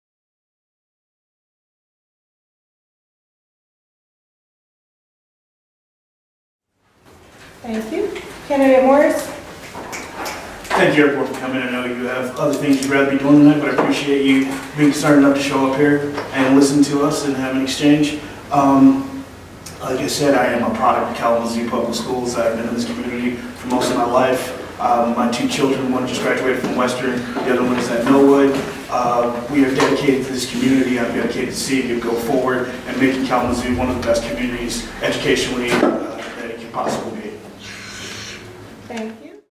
Eastside Neighborhood Association Morris Closing Statement
2918 Kalamazoo Public Schools School Board Candidate Forum